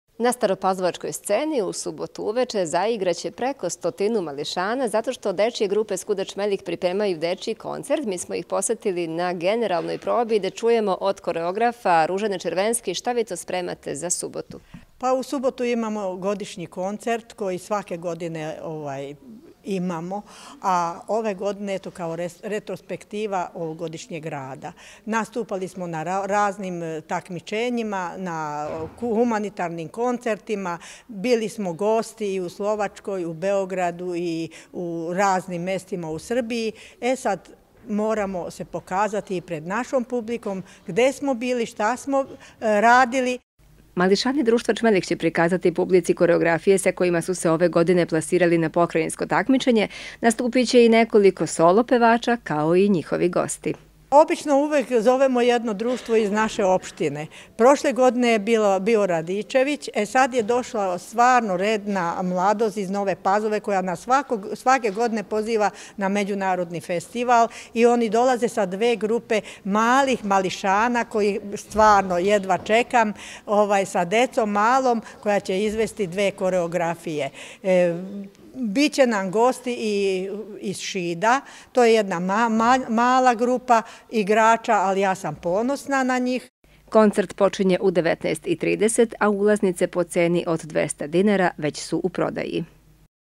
Najava dečijeg koncerta Čmelik
U sobotu uveče dečje grupe SKUD-a Čmelik iz Stare Pazove pripremaju svoj godišnji  koncert. Za ovu priliku pozvali su i goste iz Nove Pazove i Šida.